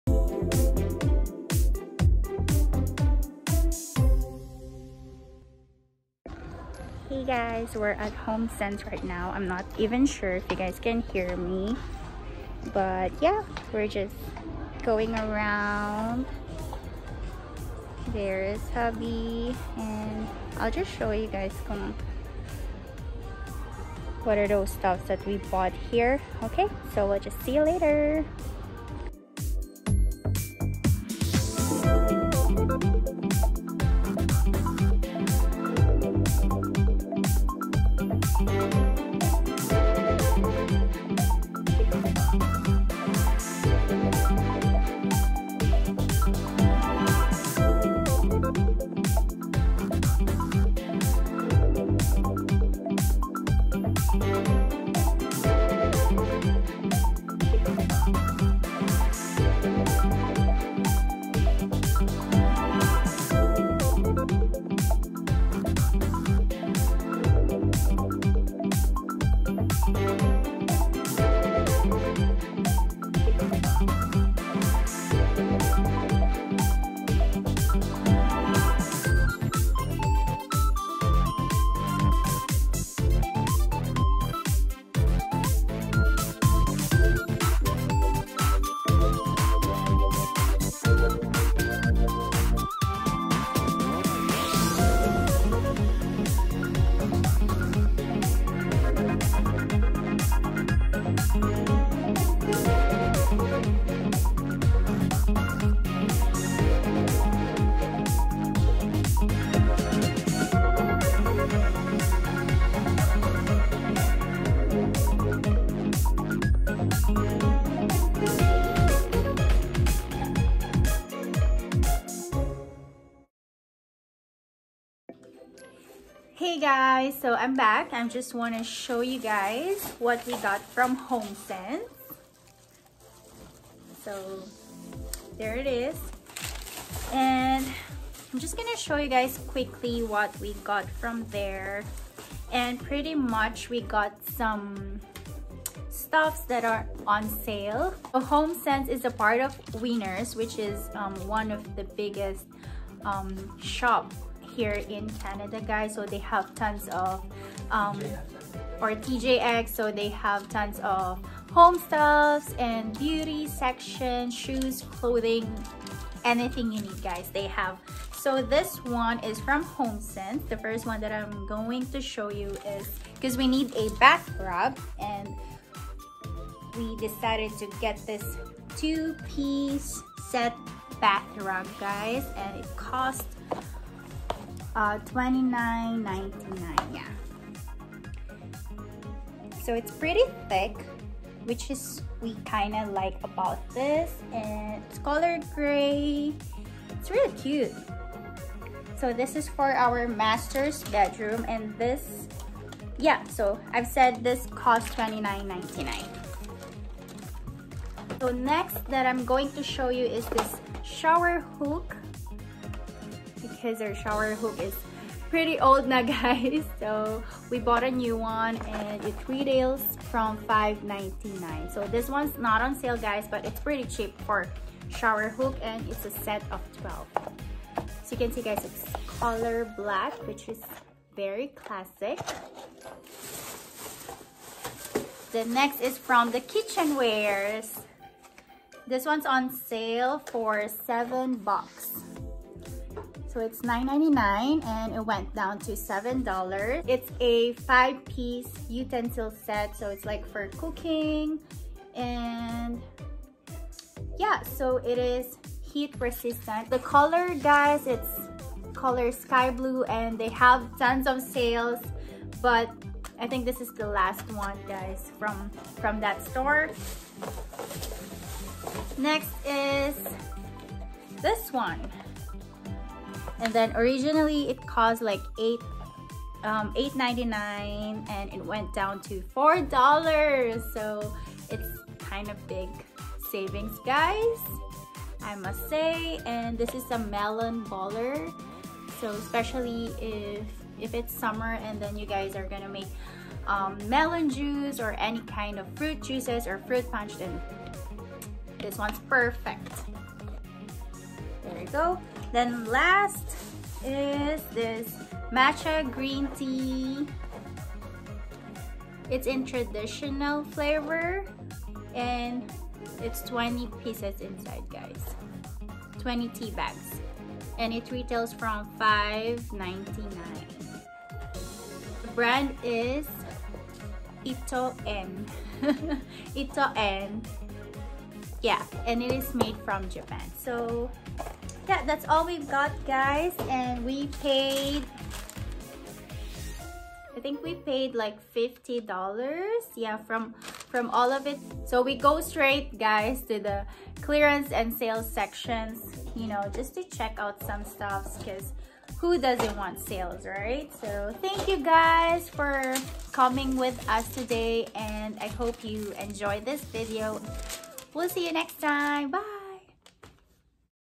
HOMESENSE & WINNERS CANADA Shopping + Haul | KITCHENWARE HAUL | Canada Vlog
We went to Homesense store here in Edmonton, Canada to look for some kitchen and home stuffs.
It is definitely one of our favorite store.